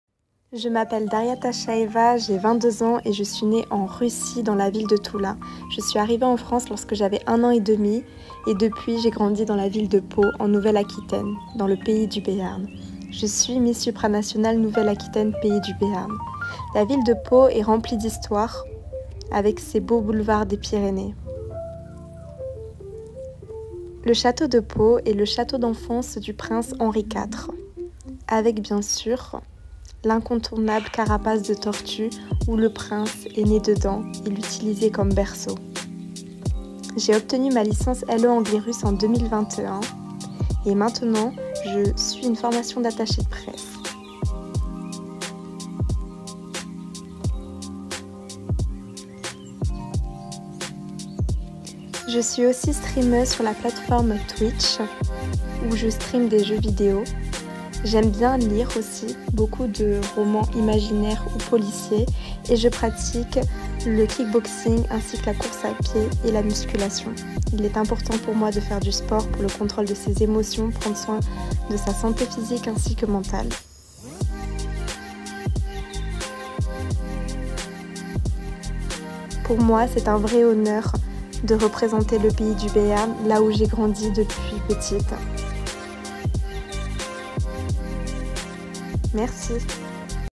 Daria TASHAEVA
Vidéo de présentation de notre candidate pour la précédente élection internationale qui s'est déroulée à Roubaix le 5 juin 2022.